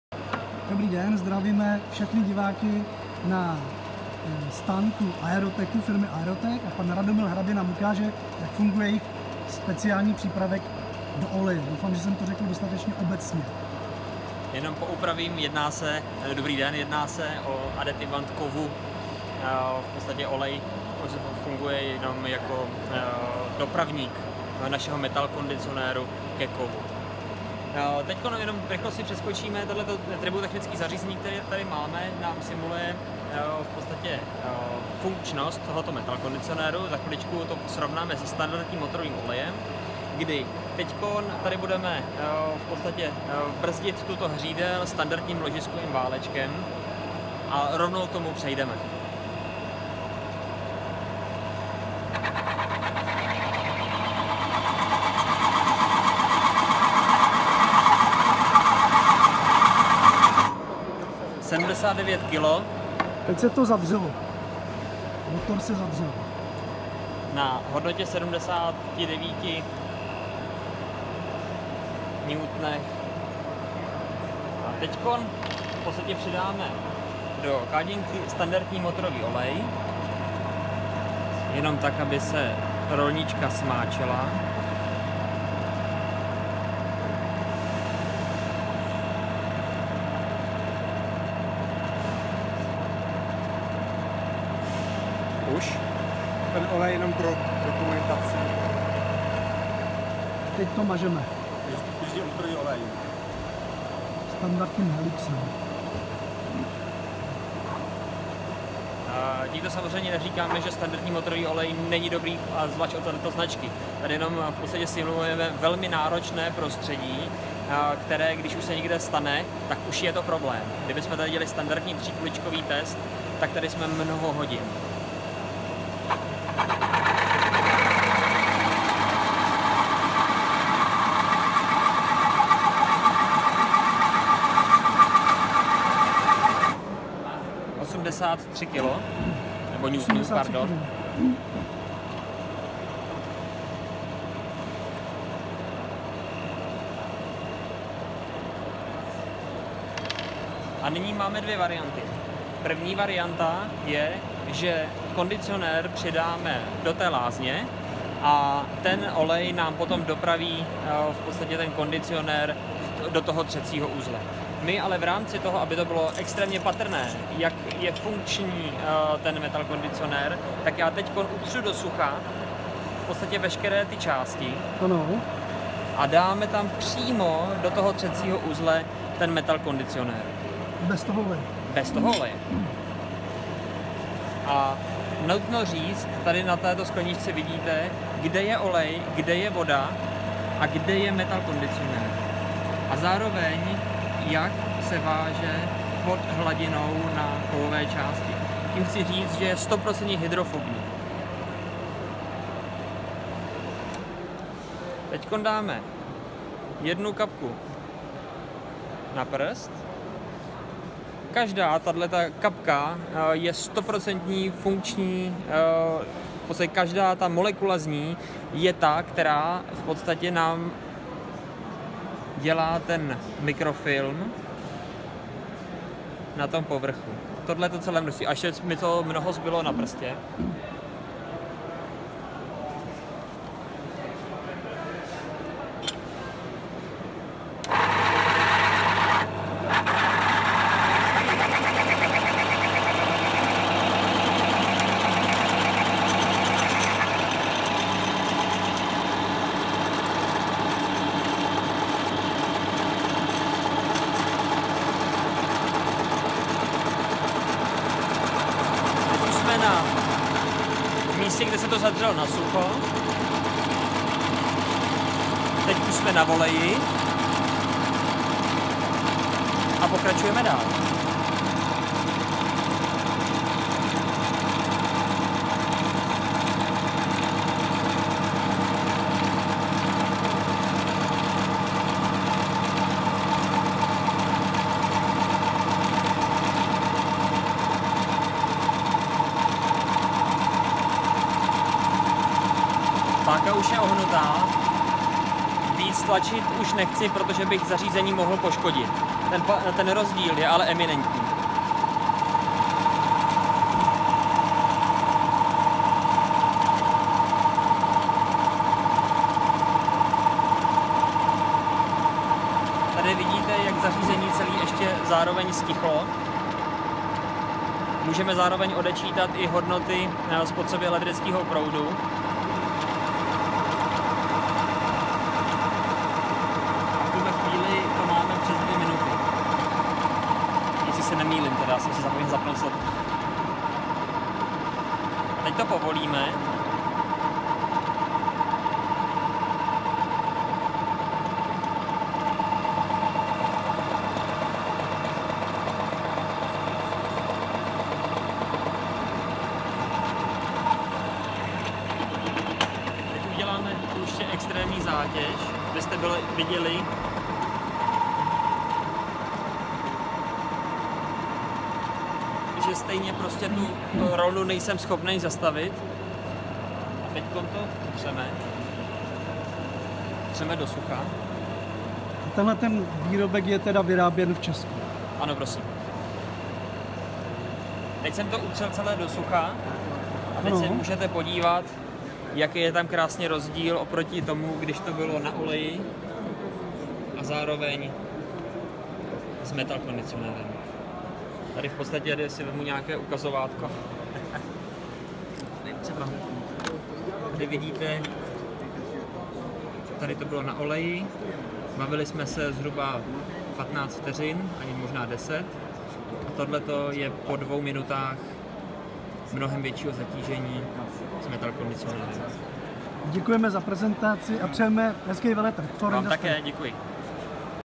Kouzelný přípravek bez kouzel - metal kondicioner
Někteří lidé používají kondicionér na vlasy, my jsme zjistili, že ČESKÁ FIRMA AEROTEC vyvinula a vyrábí kondicionér na železo :-). Rozhovor s obchodním ředitelem Radomilem Hrabě natočil Petr Smolník, šéfredaktor B2B NETWORK NEWS